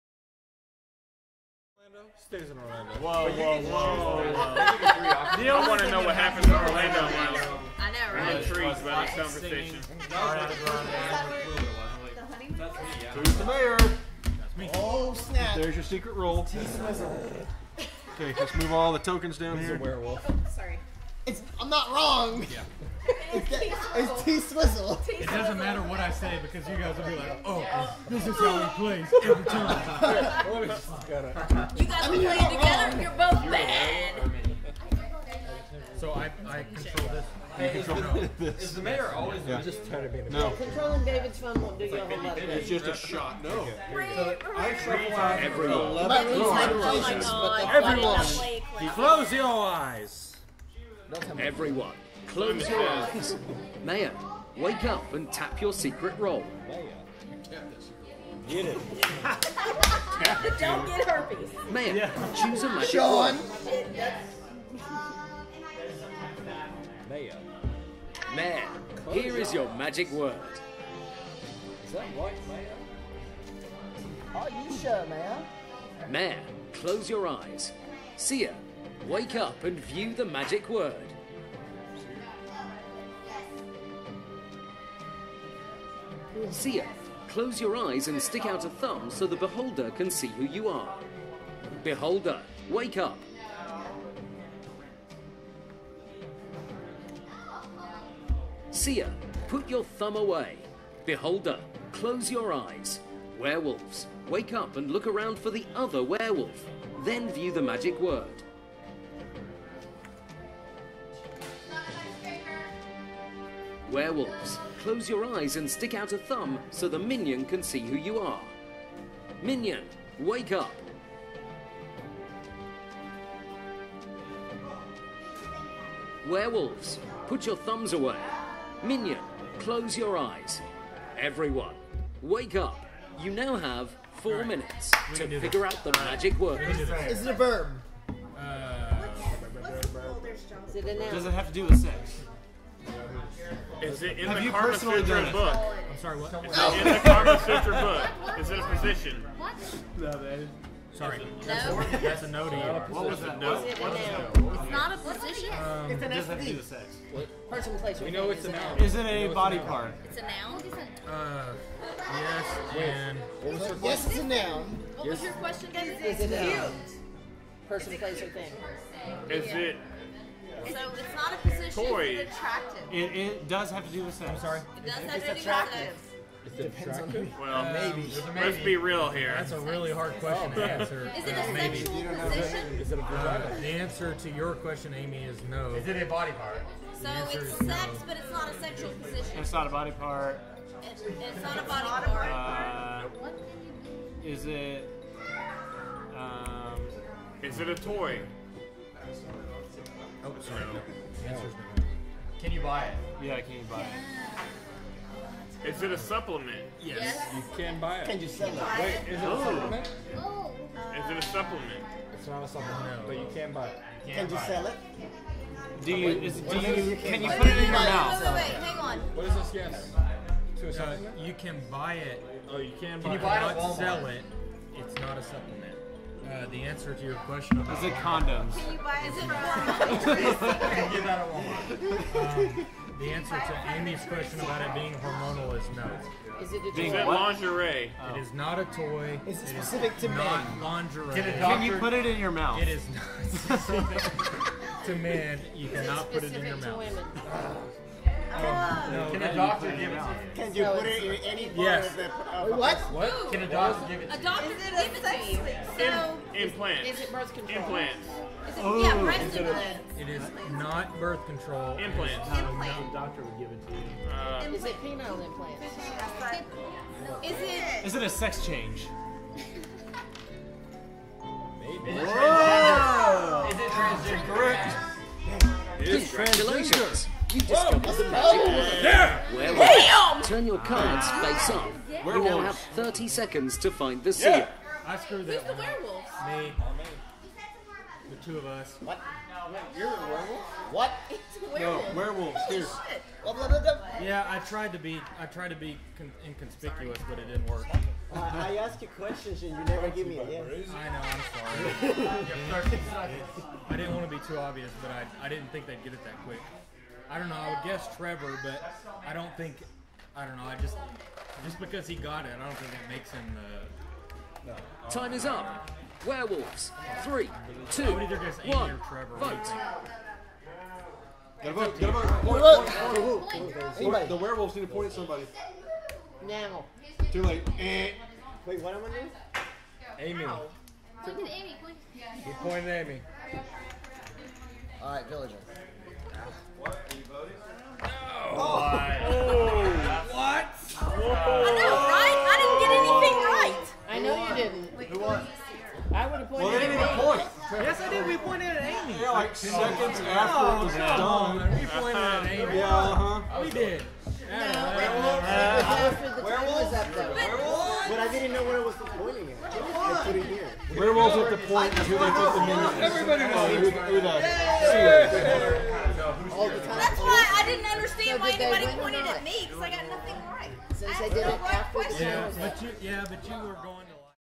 Werewords NSFW - Playthrough 8
Not Safe For Work tag due to language.  At Paper Games we discourage foul language due to children being present, but this was a private party held after hours and no children were present.

In Werewords, players guess a secret word by asking "yes" or "no" questions. Figure out the magic word before time is up, and you win! However, one of the players is secretly a werewolf who is not only working against you, but also knows the word. If you don't guess the word in time, you can still win by identifying the werewolf!

To help you out, one player is the Seer, who knows the word but must not to be too obvious when helping you figure it out; if the word is guessed, the werewolf can pull out a win by identifying the Seer!

A free iOS/Android app provides thousands of words in hundreds of categories at various difficulty levels, so everyone can play.